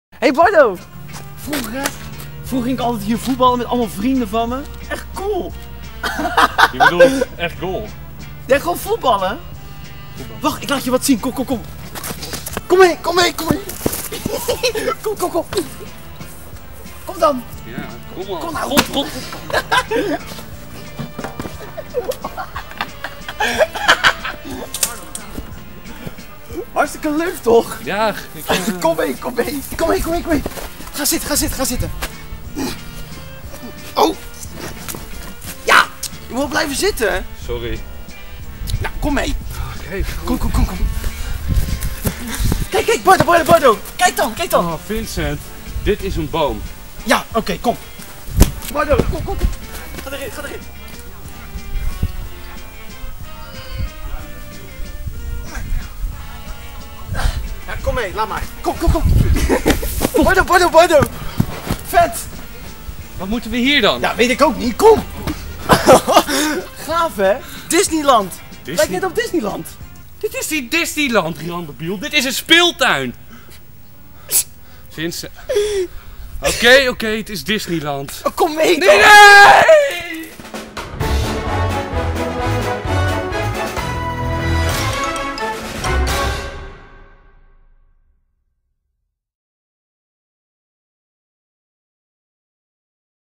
DISNEYLAND?
➤ IP DISNEYCRAFT: VINCENT.DISNEYCRAFT.NL
➤ TEAMSPEAK - VINCENT.DISNEYCRAFT.NL

[INLEIDING] - Ter ere van de Disneycraft server hebben banjomovies en ik een video gemaakt over Disneyland. Kijk zeker de stream die nu gaande is op dit kanaal!